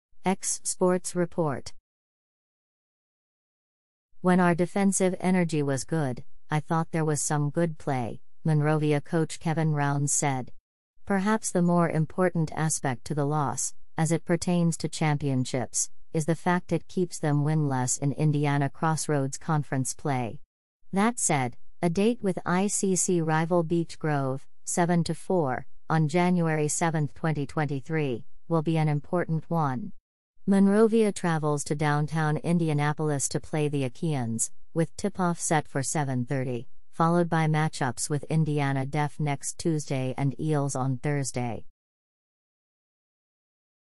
Monrovia Girls' Basketball Struggles During Matchup With Cascade
"When our defensive energy was good, I thought there was some good play," Monrovia coach Kevin Rounds said. Perhaps the more important aspect to the loss, as it pertains to championships, is the fact it keeps them winless in Indiana Crossroads Conference play. That said, a date with ICC rival Beech Grove (7-4) on Jan. 7, 2023, will be an important one. Monrovia travels to downtown Indianapolis to play the Achaeans, with tip-off set for 7:30, followed by matchups with Indiana Deaf next Tuesday and Eels on Thursday.